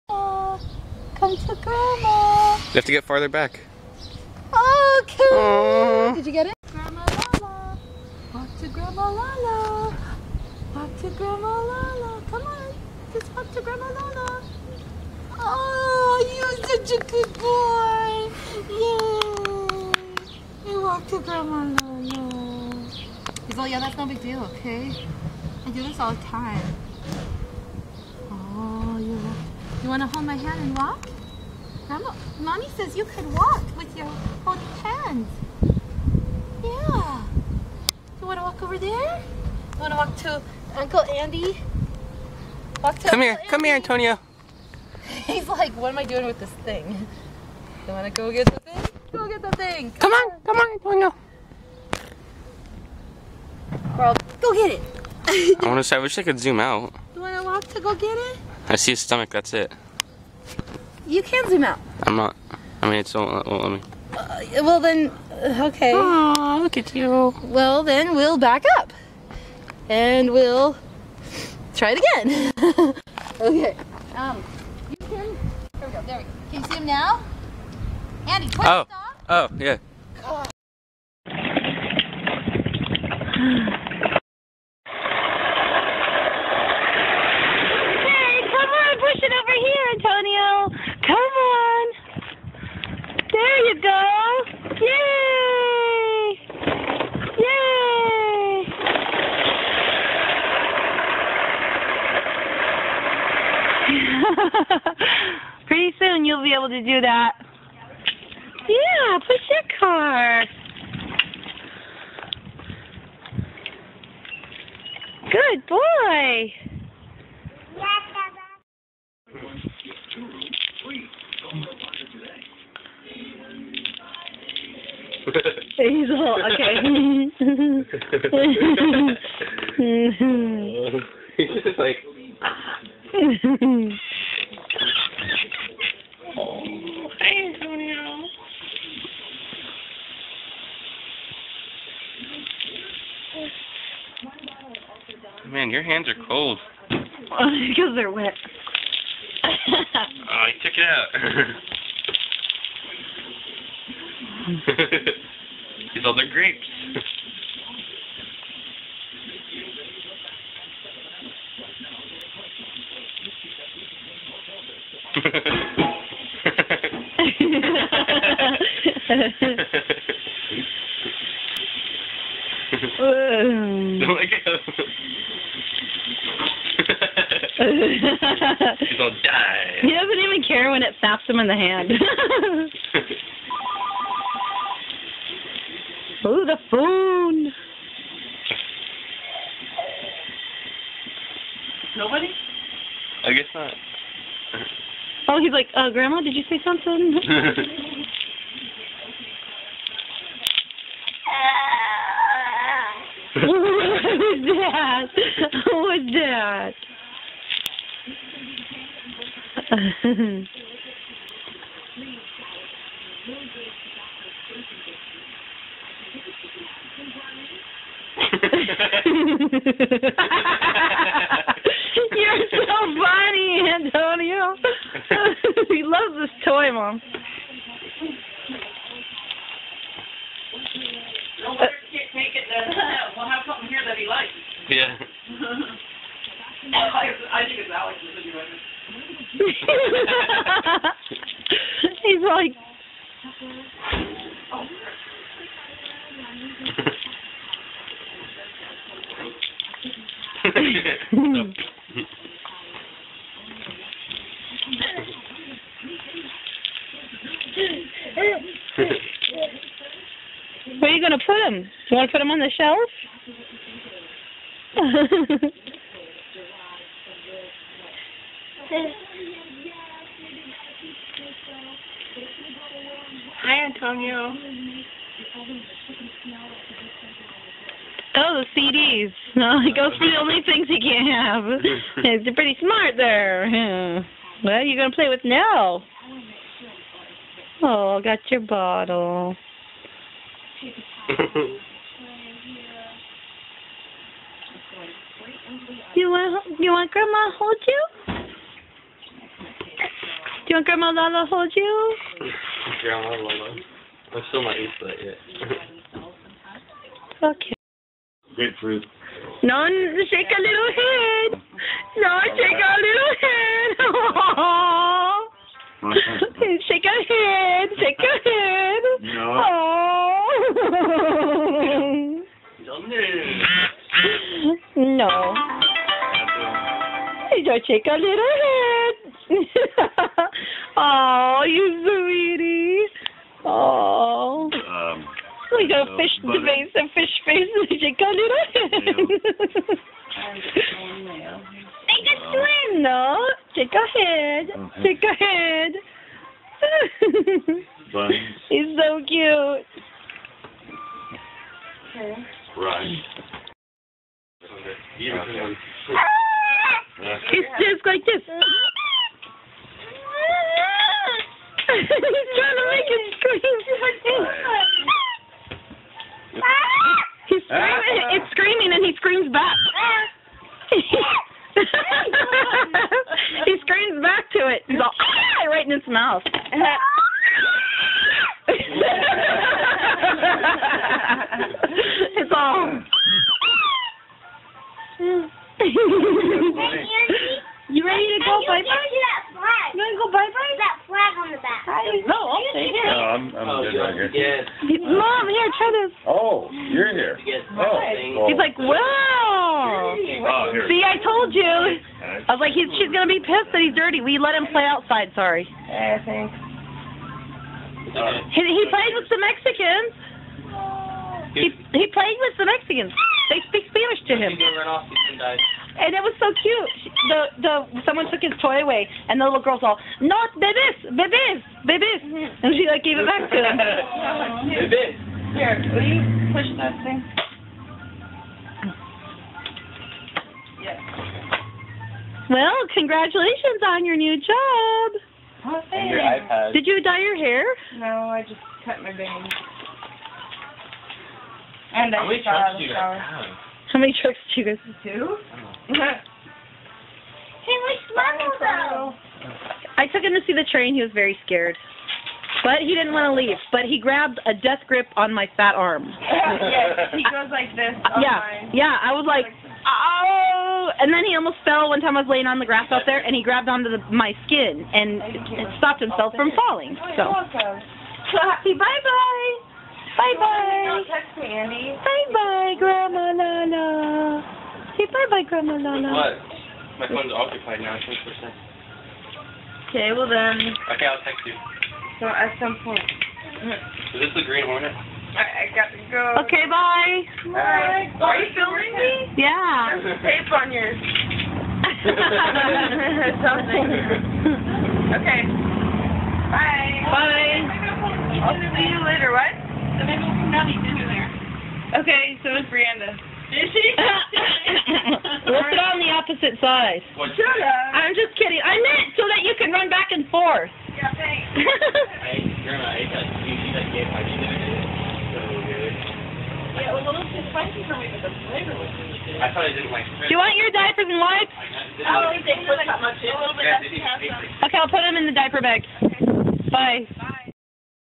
Antonio and Grandma Lala July 2011
Heather and Andy babysitting Antonio for a couple hours in July, 2011
He has just learned how to take a few steps on his own so he walks to grandma Lala (Heather).
He pushes a blue car across the carport, puts a Kush Ball in his mouth,
Shakes-A-Little-Head and screams back at a squeaky toy.
Then Cassandra comes and picks him up and takes him to her house.

Antonio is 11 months old in this video.